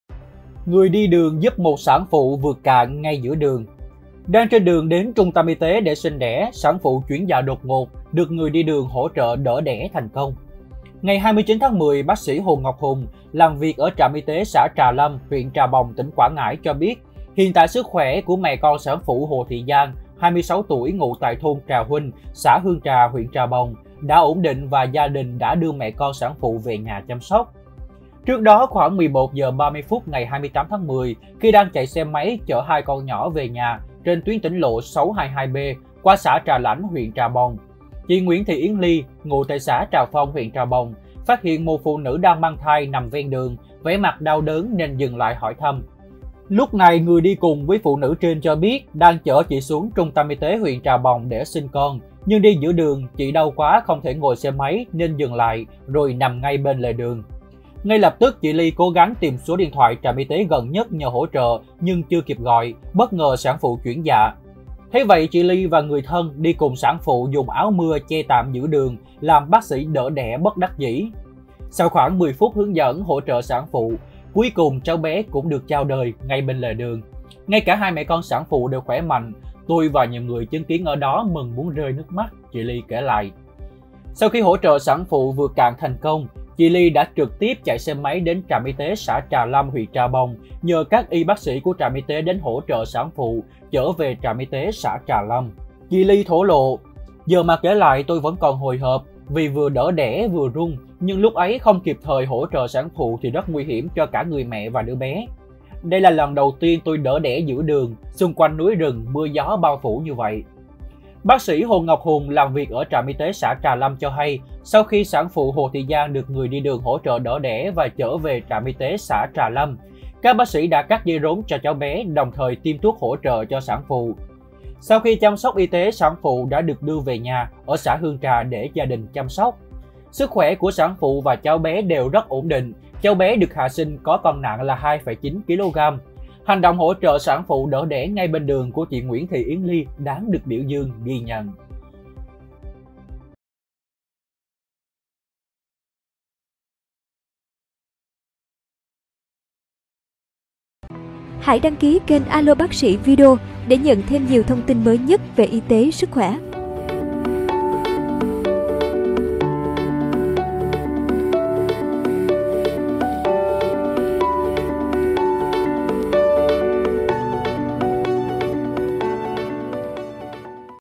Người đi đường giúp một sản phụ "vượt cạn" ngay giữa đường
Đang trên đường đến Trung tâm y tế để sinh đẻ, sản phụ chuyển dạ đột ngột được người đi đường hỗ trợ đỡ đẻ thành công.👉 Tin tức COVID-19 mới nhất

MỌI GÓP Ý XIN GỬI VỀ:
Hợp tác nội dung:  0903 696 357